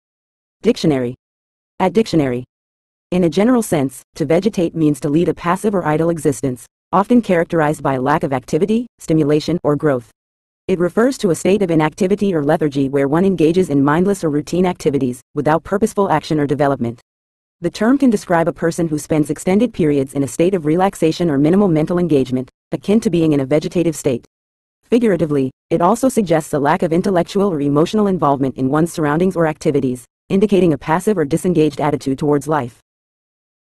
Vegetate Meaning In English
In a general sense, to vegetate means to lead a passive or idle existence, often characterized by a lack of activity, stimulation, or growth. It refers to a state of inactivity or lethargy where one engages in mindless or routine activities, without purposeful action or development. The term can describe a person who spends extended periods in a state of relaxation or minimal mental engagement, akin to being in a vegetative state. Figuratively, it also suggests a lack of intellectual or emotional involvement in one's surroundings or activities, indicating a passive or disengaged attitude towards life.